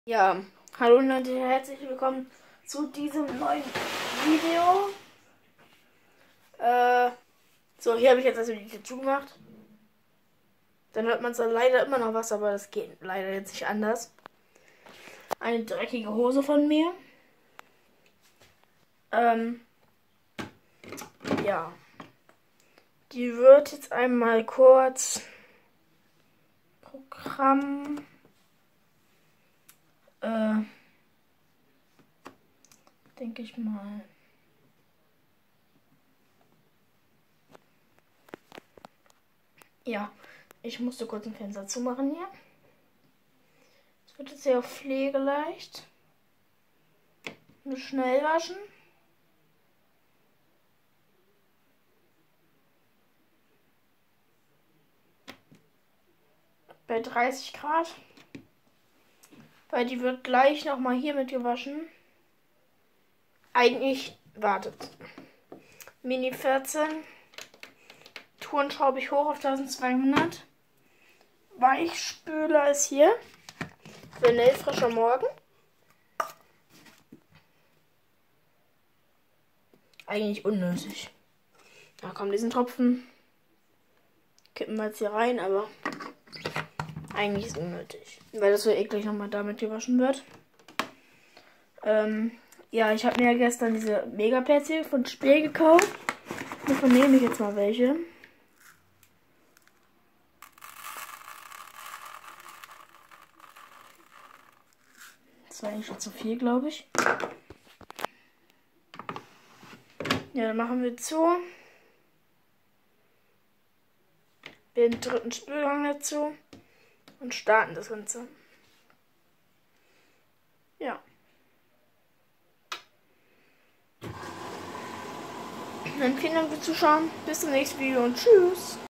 Grundig Waschmaschine
Hier mal eine dreckige Hose auf Mini 14 und 1200 Touren.
Ich habe sie am Ende doch nicht Schleudern lassen,da sie Probleme mit der UWK hatte.